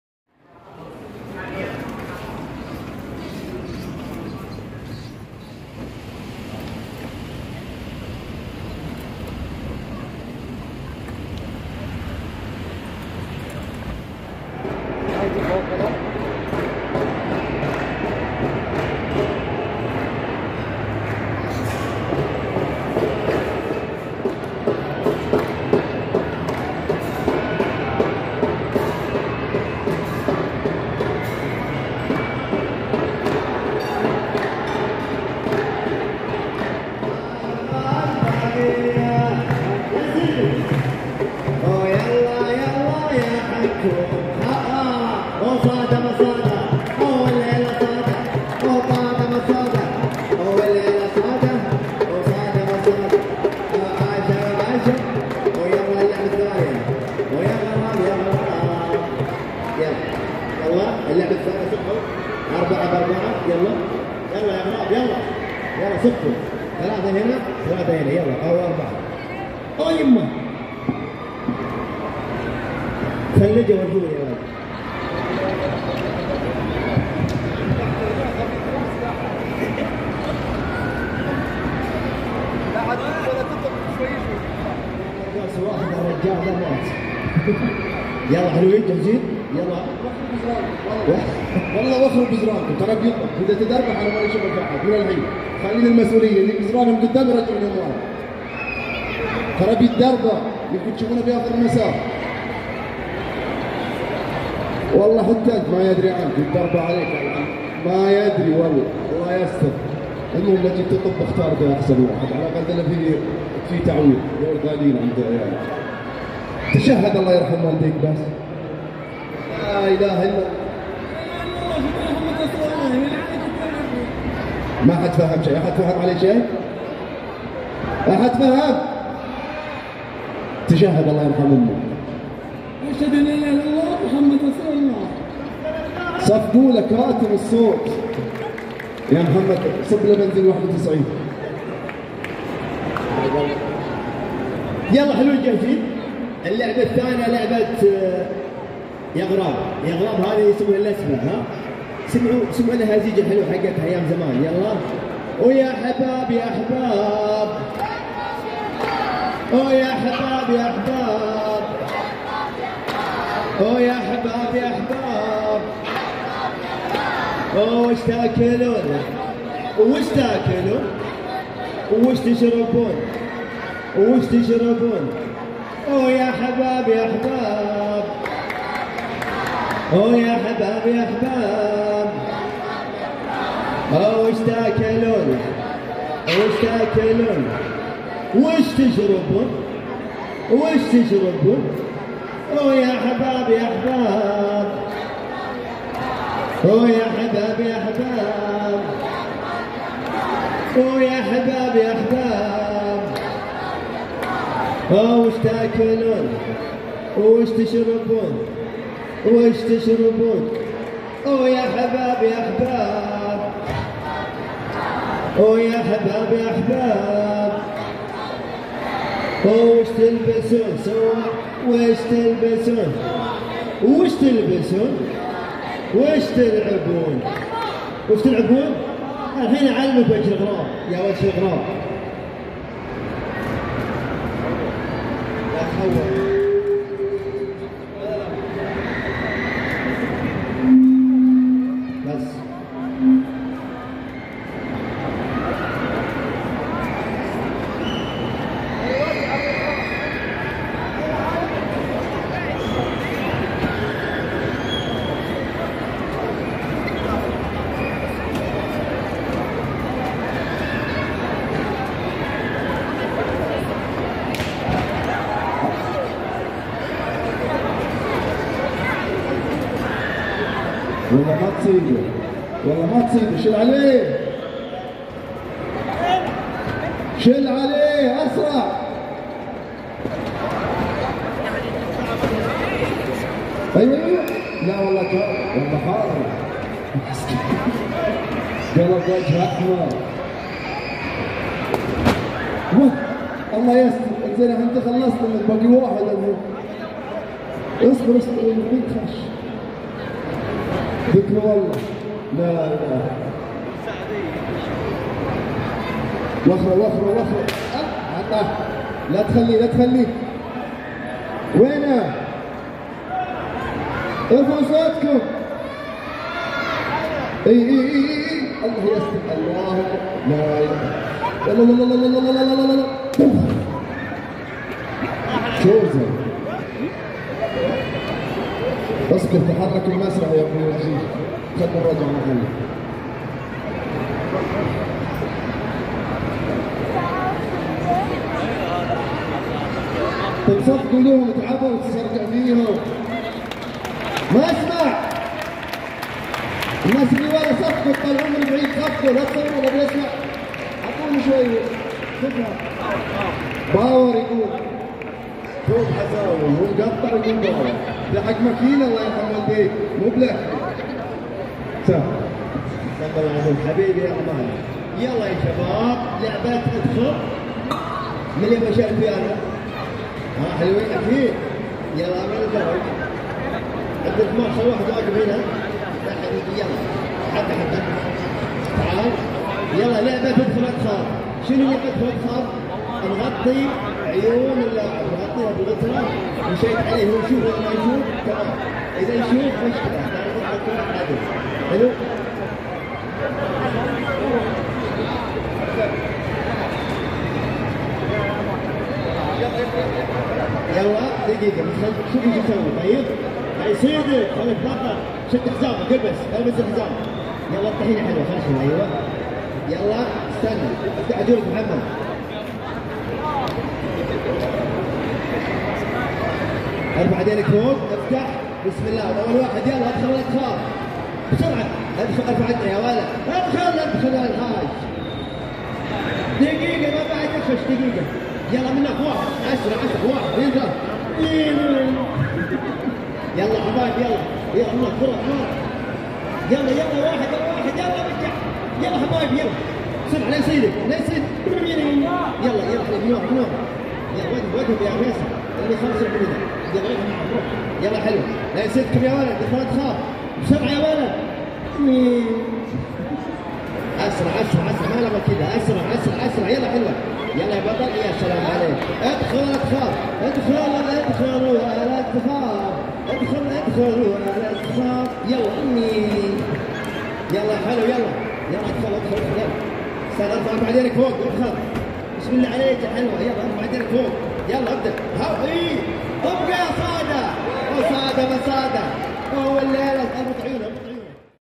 The Day before Ramadan April 1 2022 at Othaim Mall Mubarraz Al Hassa Saudi Arabia | Ramadan Mubarak
Happy Ramadan Mubarak!
For more videos and recipes